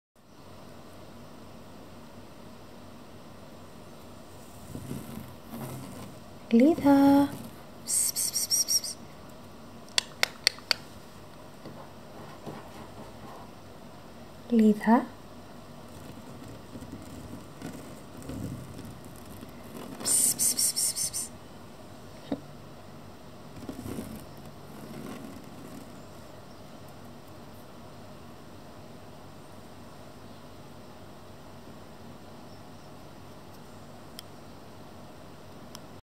GR*Elvenstar's Leda 37 days old
Our sweet Norwegian Forest princess is growing and becoming more beautiful by the day!More photos and information about Leda and the rest of the "Jupiter" litter